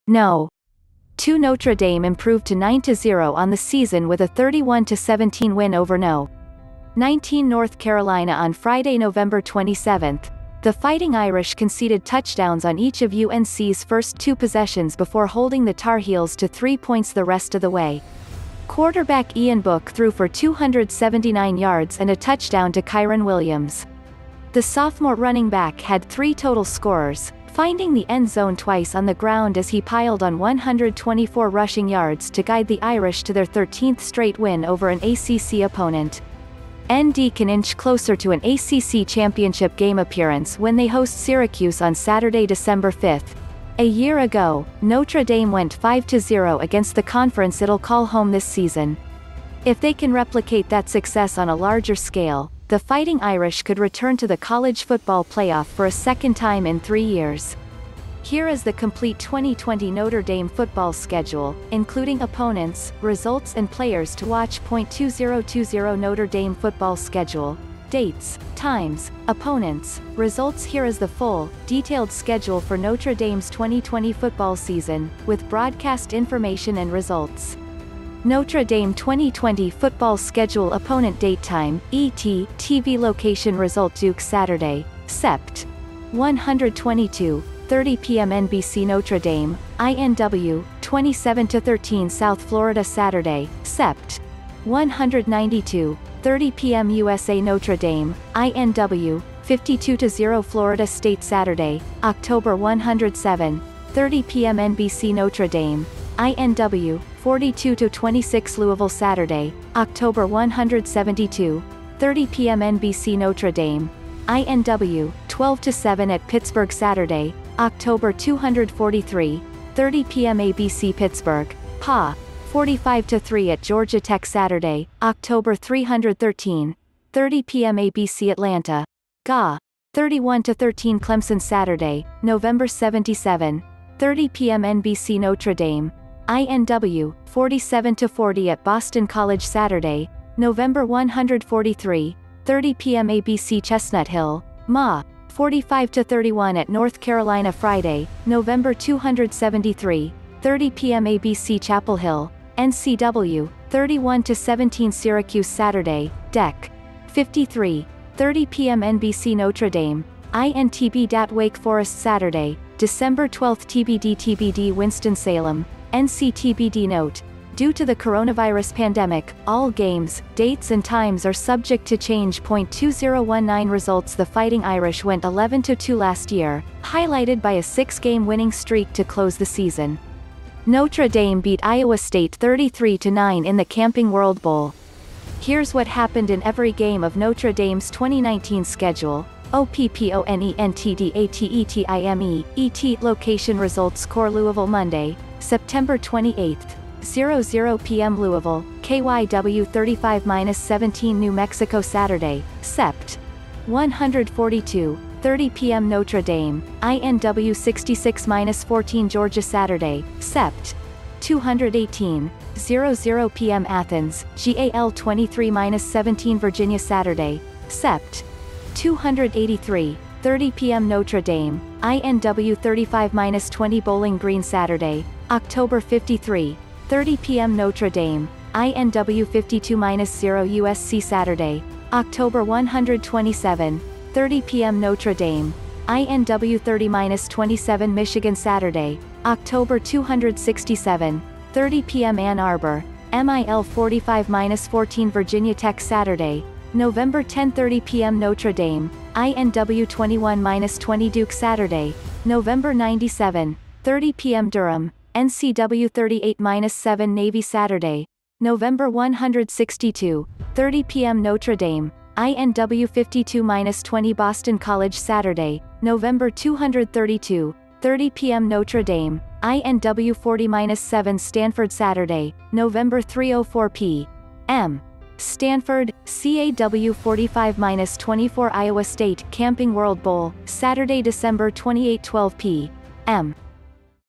2020 Notre Dame football schedule Dates times opponents results
No. 2 Notre Dame improved to 9-0 on the season with a 31-17 win over No. 19 North Carolina on Friday, Nov. 27. The Fighting Irish conceded touchdowns on each of UNC's first two possessions before holding the Tar Heels to three points the rest of the way. Quarterback Ian Book threw for 279 yards and a touchdown to Kyren Williams. The sophomore running back had three total scorers, finding the end zone twice on the ground as he piled on 124 rushing yards to guide the Irish to their 13th straight win over an ACC opponent. ND can inch closer to an ACC Championship Game appearance when they host Syracuse on Saturday, Dec. 5. A year ago, Notre Dame went 5-0 against the conference it'll call home this season. If they can replicate that success on a larger scale, the Fighting Irish could return to the College Football Playoff for a second time in three years. Here is the complete 2020 Notre Dame football schedule, including opponents, results and players to watch.2020 Notre Dame football schedule: Dates, times, opponents, resultsHere is the full, detailed schedule for Notre Dame's 2020 football season, with broadcast information and results. Notre Dame 2020 Football ScheduleOpponentDateTime (ET)TVLocationRESULTDukeSaturday, Sept. 122:30 p.m. NBCNotre Dame, INW, 27-13South FloridaSaturday, Sept. 192:30 p.m. USANotre Dame, INW, 52-0Florida StateSaturday, Oct. 107:30 p.m. NBCNotre Dame, INW, 42-26LouisvilleSaturday, Oct. 172:30 p.m. NBCNotre Dame, INW, 12-7at PittsburghSaturday, Oct. 243:30 p.m. ABCPittsburgh, PAW, 45-3at Georgia TechSaturday, Oct. 313:30 p.m. ABCAtlanta, GAW, 31-13ClemsonSaturday, Nov. 77:30 p.m. NBCNotre Dame, INW, 47-40at Boston CollegeSaturday, Nov. 143:30 p.m. ABCChestnut Hill, MAW, 45-31at North CarolinaFriday, Nov. 273:30 p.m. ABCChapel Hill, NCW, 31-17SyracuseSaturday, Dec. 53:30 p.m. NBCNotre Dame, INTBDat Wake ForestSaturday, Dec. 12TBDTBDWinston-Salem, NCTBDNote: Due to the coronavirus pandemic, all games, dates and times are subject to change.2019 resultsThe Fighting Irish went 11-2 last year, highlighted by a six-game winning streak to close the season. Notre Dame beat Iowa State 33-9 in the Camping World Bowl. Here’s what happened in every game of Notre Dame's 2019 schedule:OPPONENTDATETIME (ET)LOCATIONRESULTSCORELouisvilleMonday, Sept. 28:00 p.m. Louisville, KYW35-17New MexicoSaturday, Sept. 142:30 p.m. Notre Dame, INW66-14GeorgiaSaturday, Sept. 218:00 p.m. Athens, GAL23-17VirginiaSaturday, Sept. 283:30 p.m. Notre Dame, INW35-20Bowling GreenSaturday, Oct. 53:30 p.m. Notre Dame, INW52-0USCSaturday, Oct. 127:30 p.m. Notre Dame, INW30-27MichiganSaturday, Oct. 267:30 p.m. Ann Arbor, MIL45-14Virginia TechSaturday, Nov. 22:30 p.m. Notre Dame, INW21-20DukeSaturday, Nov. 97:30 p.m. Durham, NCW38-7NavySaturday, Nov. 162:30 p.m. Notre Dame, INW52-20Boston CollegeSaturday, Nov. 232:30 p.m. Notre Dame, INW40-7StanfordSaturday, Nov. 304 p.m. Stanford, CAW45-24Iowa State(Camping World Bowl)Saturday, Dec. 2812 p.m.